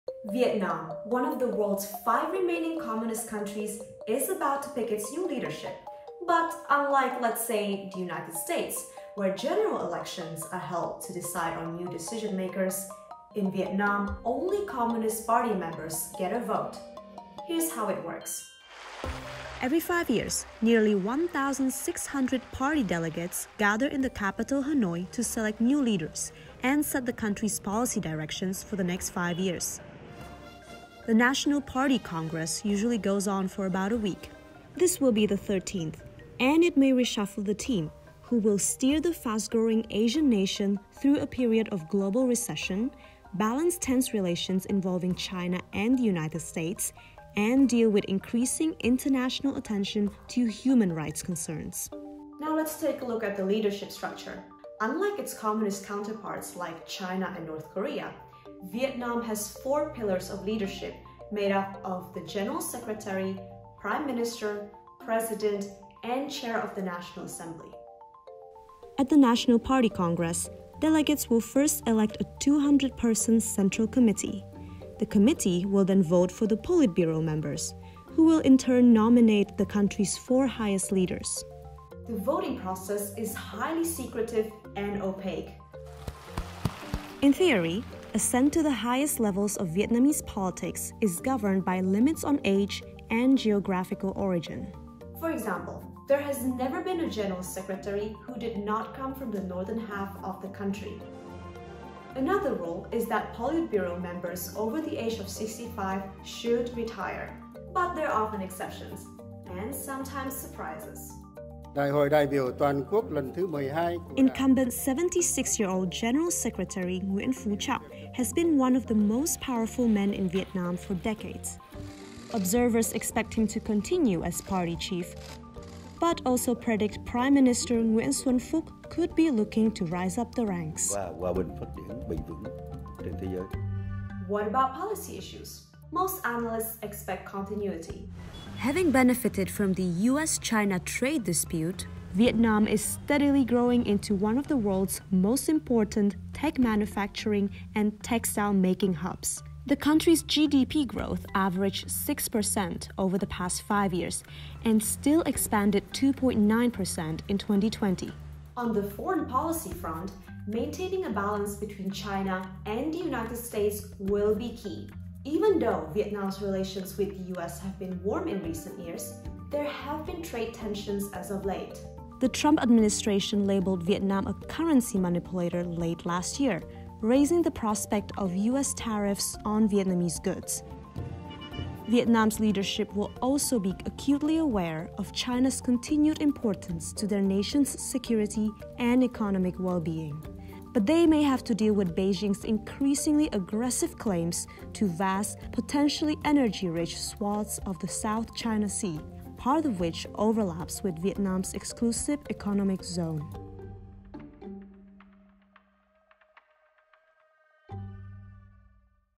Explainer: What happens at Vietnam's Communist congress?
Vietnam – one of the world's five remaining communist countries, is about to pick its new leaders. But only Communist Party members get a vote. Lucy Ha explains what happens at Vietnam's Communist congress